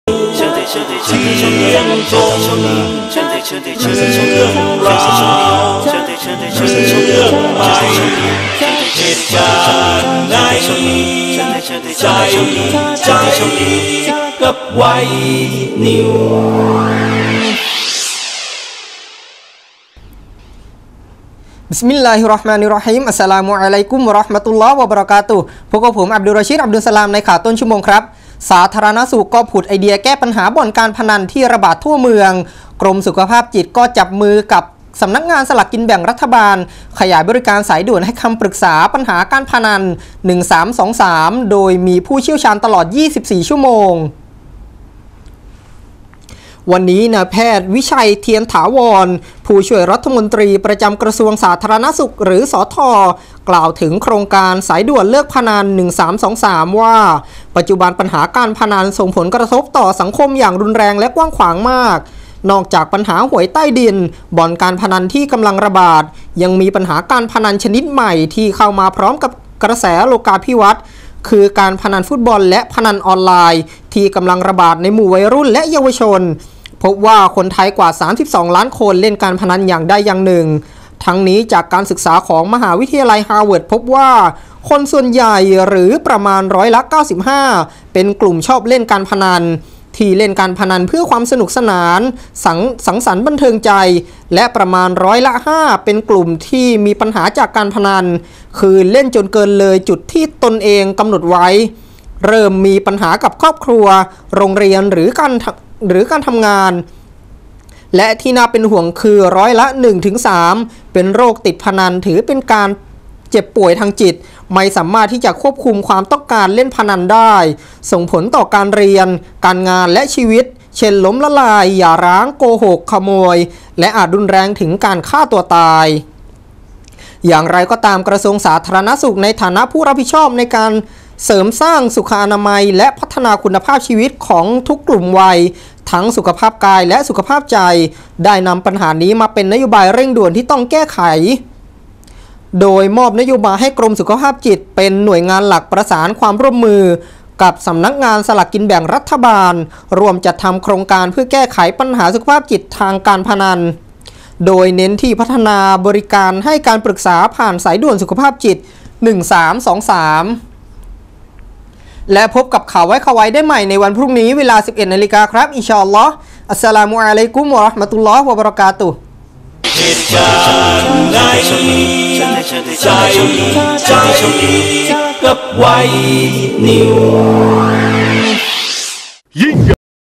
สธ.ผุด "สายด่วนเลิกเล่นพนัน 1323"
สาธารณสุข ผุดไอเดียแก้ไขปัญหาบ่อนการพนันระบาดทั่วเมือง กรมสุขภาพจิตจับมือสำนักงานสลากกินแบ่งรัฐบาล ขยายบริการสายด่วนให้การปรึกษาปัญหาพนัน 1323 โดยผู้เชี่ยวชาญตลอด 24 ชั่วโมง

            วันนี้ นพ.วิชัย เทียนถาวร ผู้ช่วยรัฐมนตรีประจำกระทรวงสาธารณสุข (สธ.) กล่าวถึงโครงการ "สายด่วนเลิกพนัน 1323" ว่า ปัจจุบันปัญหาการพนัน ส่งผลกระทบต่อสังคมอย่างรุนแรงและกว้างขวางมาก ซึ่งนอกจากปัญหาหวยใต้ดิน บ่อนการพนันที่กำลังระบาด ยังมีปัญหาการพนันชนิดใหม่ที่เข้ามาพร้อมกับกระแส โลกาภิวัตน์ คือการพนันฟุตบอลและพนันออนไลน์ที่กำลังระบาดในหมู่วัยรุ่นและเยาวชน พบว่า คนไทยกว่า 32 ล้านคน เล่นการพนันอย่างใดอย่างหนึ่ง

 ทั้งนี้ จากการศึกษาของมหาวิทยาลัยฮาร์วาร์ด พบว่า คนส่วนใหญ่หรือประมาณร้อยละ 95 เป็นกลุ่มชอบเล่นพนัน (Social Gambling) ที่เล่นการพนันเพื่อความสนุกสนาน สังสรรค์บันเทิงใจ และประมาณร้อยละ 5 เป็นกลุ่มมีปัญหาจากพนัน (Problem Gambling) คือเล่นจนเกินเลยจุดที่ตนกำหนดไว้ เริ่มมีปัญหากับครอบครัว โรงเรียนหรือการงาน และที่น่าเป็นห่วงคือ ร้อยละ 1-3 เป็นโรคติดพนันถือเป็นการเจ็บป่วยทางจิต ไม่สามารถ ควบคุมความต้องการเล่นพนันได้ ส่งผลต่อการเรียน การงานและชีวิต เช่น ล้มละลาย หย่าร้าง โกหก ขโมยและอาจรุนแรงถึงการฆ่าตัวตาย

 อย่างไรก็ตาม กระทรวงสาธารณสุข ในฐานะผู้รับผิดชอบในการสร้างเสริมสุขอนามัยและพัฒนาคุณภาพชีวิตของทุกกลุ่มวัย ทั้งสุขภาพกายและสุขภาพใจ ได้นำปัญหานี้มาเป็นนโยบายเร่งด่วนที่ต้องรีบแก้ไข โดยมอบนโยบายให้กรมสุขภาพจิต เป็นหน่วยงานหลักประสานความร่วมมือกับสำนักงานสลากกินแบ่งรัฐบาล ร่วมจัดทำ โครงการเพื่อแก้ไขปัญหาสุขภาพจิตจากการพนัน โดยเน้นที่พัฒนาบริการให้การปรึกษาผ่าน "สายด่วนสุขภาพจิต 1323"